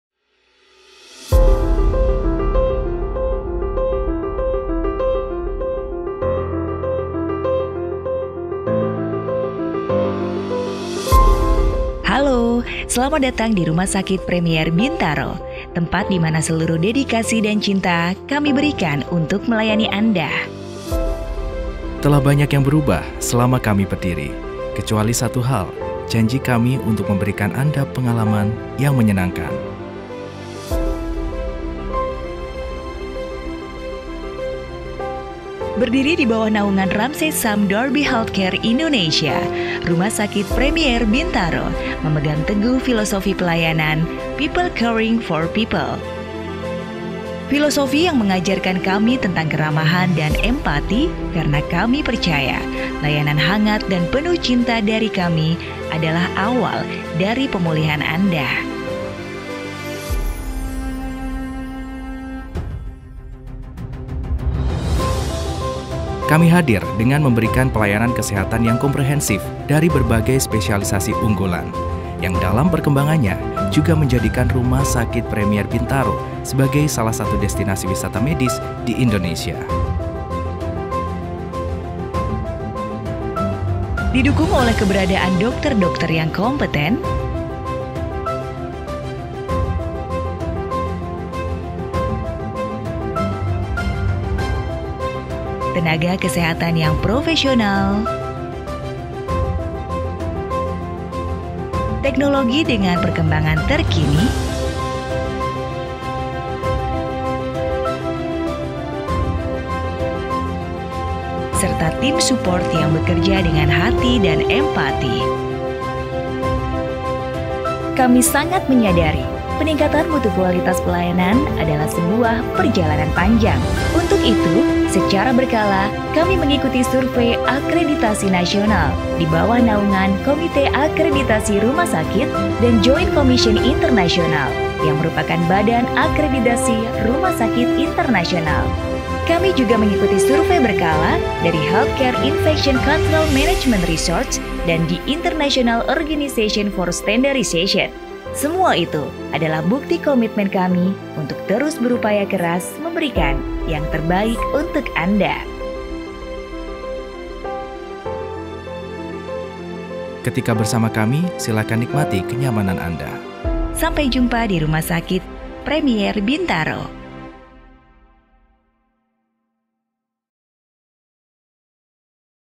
Company Profile RS Premier Bintaro: Your Experience is Our Priority
Begitu banyak yang berubah selama hampir 25 tahun RS Premier Bintaro berdiri. Kecuali satu hal, yakni janji kami untuk tetap memberikan Anda pelayanan terbaik dengan dengan cinta dan empati.